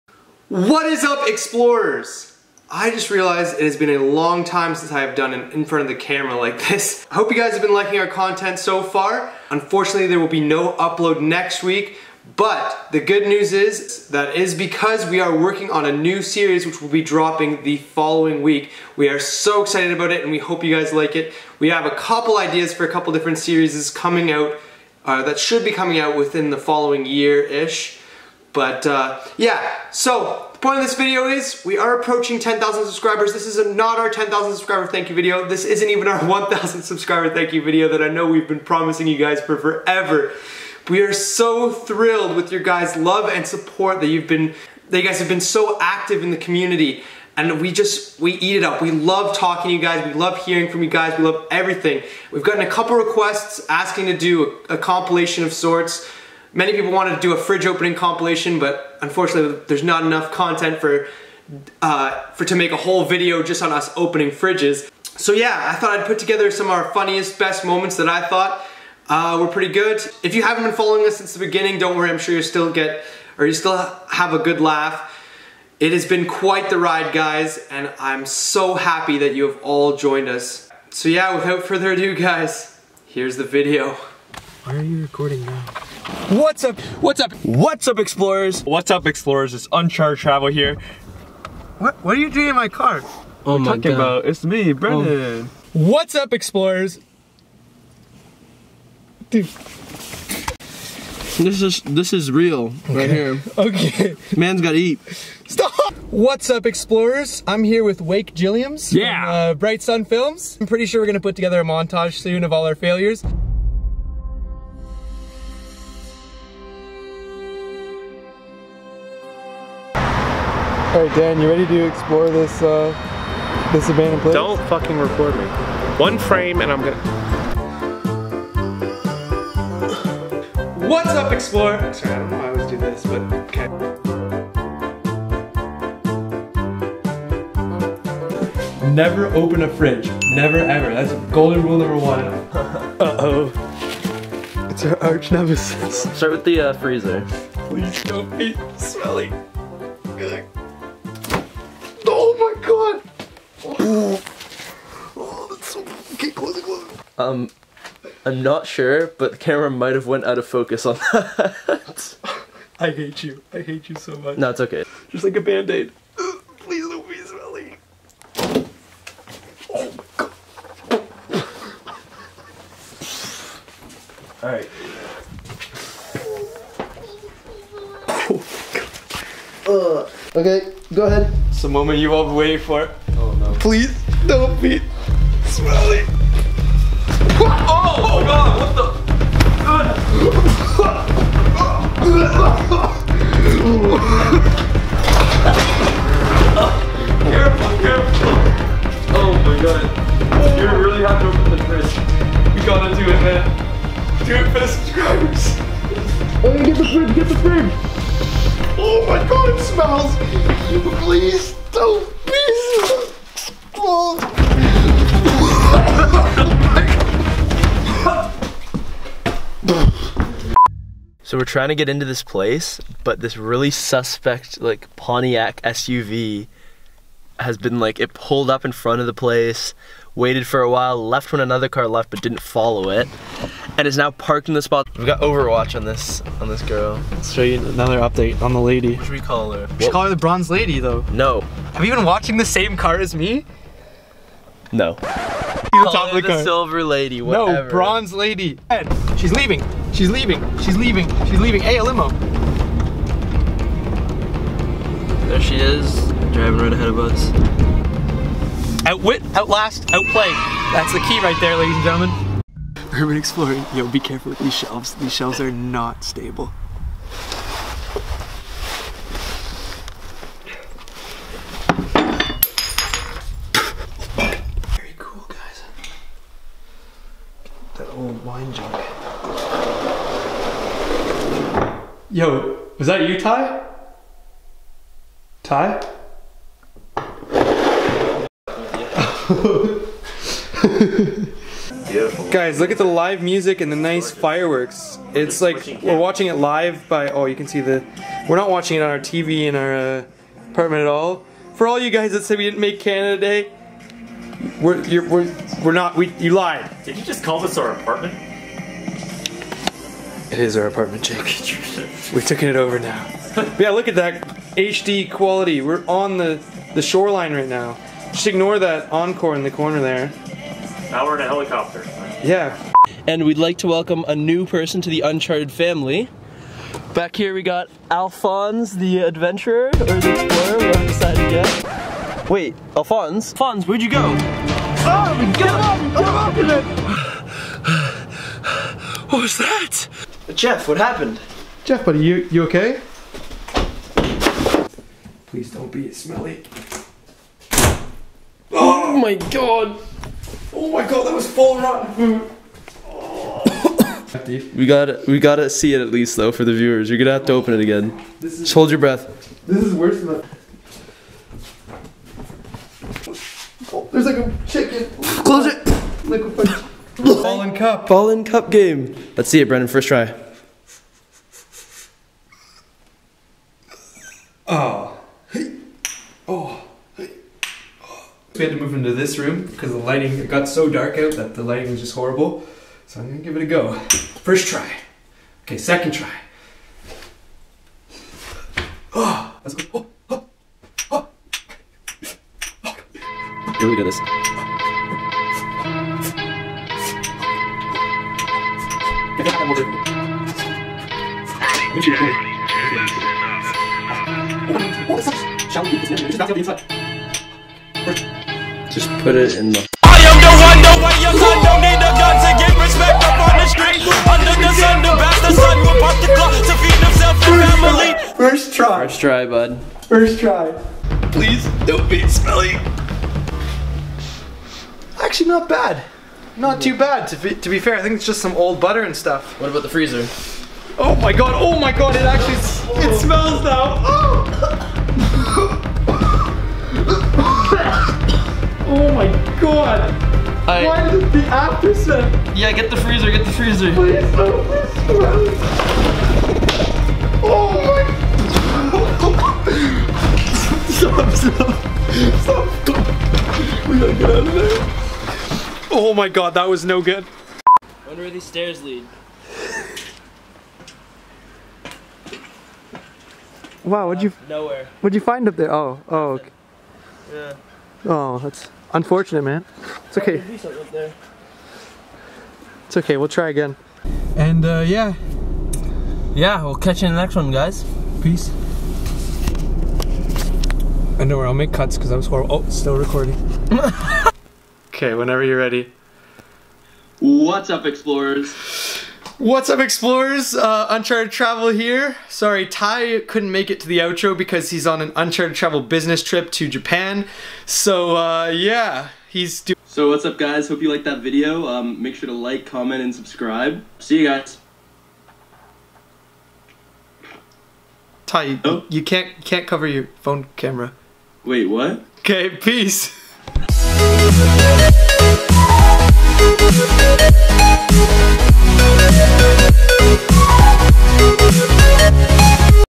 Uncharted Travel's Best / Funniest Moments Compilation
As we approach 1 year of being on Youtube this October, we here at Uncharted Travel thought it would be fun to put together a compilation of all our best and worst moments, fails, skits, and funny memes. Join us as we take a look back at some of the worst and best content ever created on this channel.

If you enjoyed this video and want to see some awesome abandoned places memes like these, don't forget to:

Thank you so much to everyone who has helped us and explored with us along this crazy journey! Catch you guys in the next one!